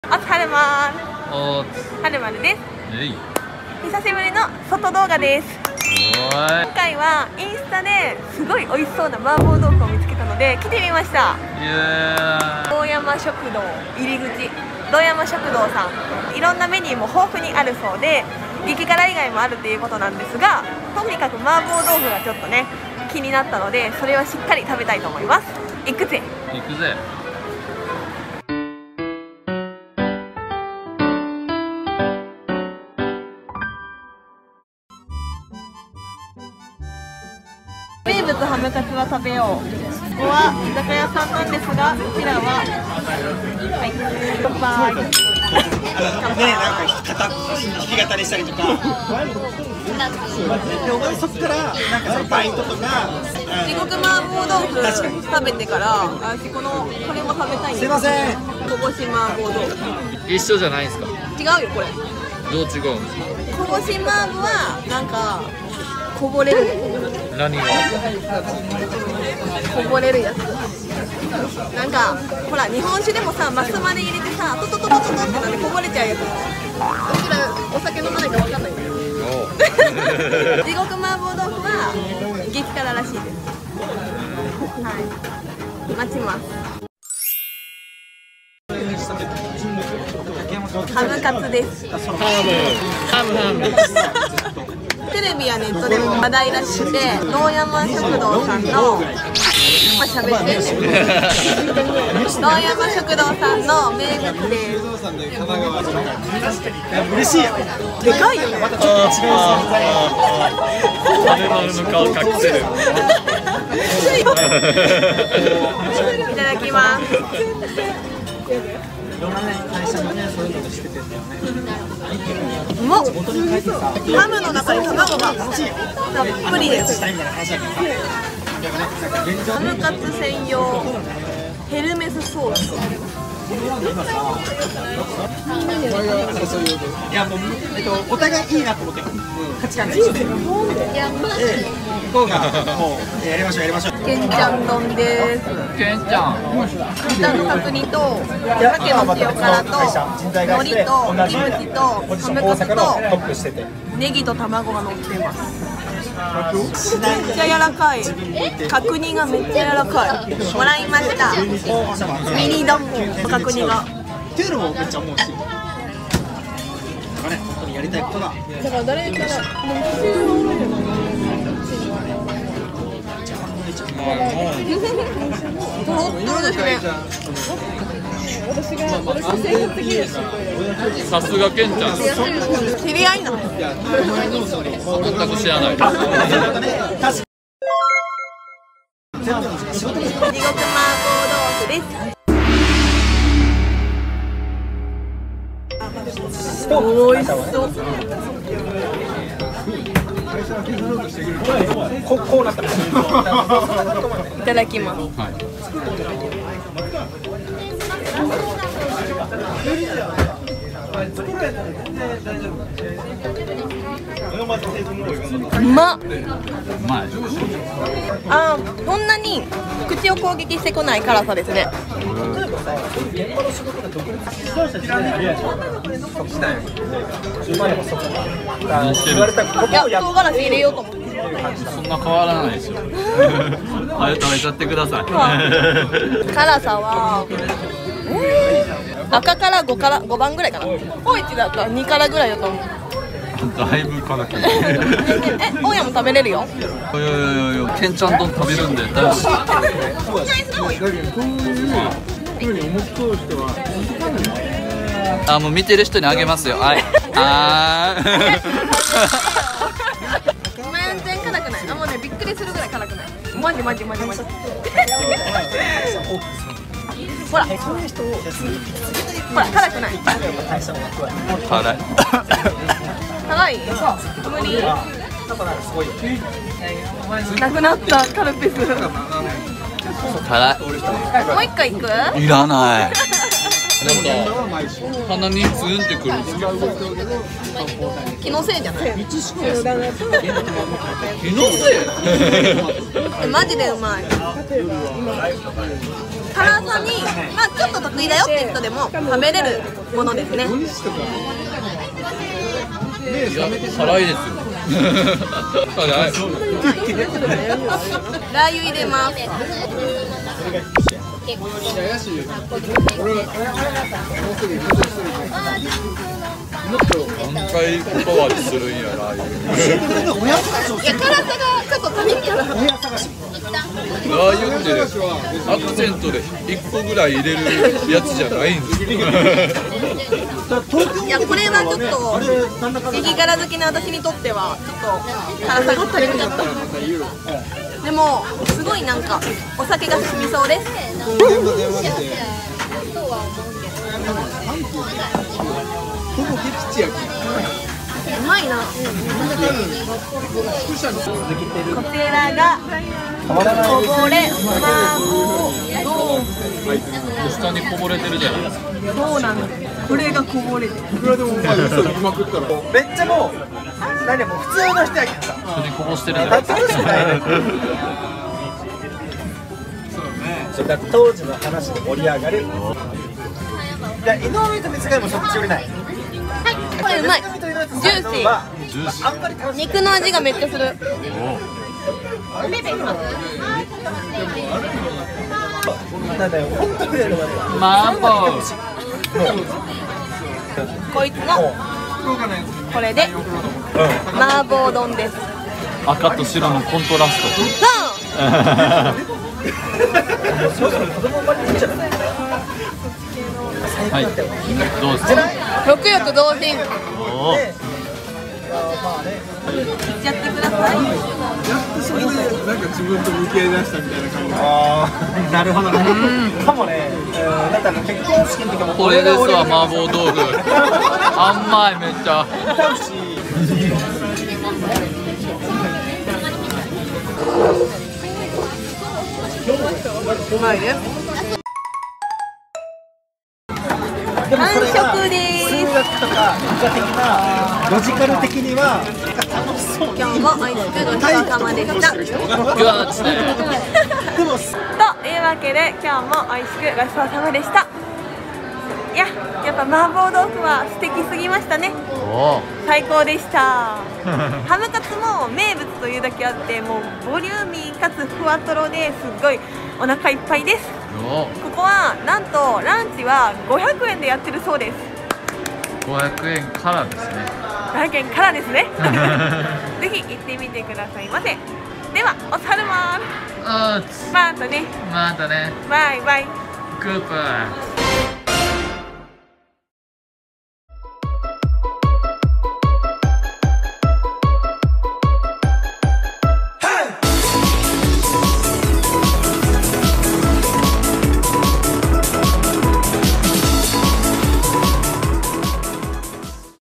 【唐辛子地獄】地獄の麻婆豆腐で唐辛子まみれになった【堂山食堂】
本日の動画は、音声乱れてますm(_ _)m申し訳ありません。

唐辛子まみれ🌶🌶🌶🌶

【本日のお店】

堂山食堂　東梅田（食べログ）

はるまるコンテンツ(*´꒳`*)

今後とも、よろしくお願いします♬
ーーーーーーーーーーーーーーー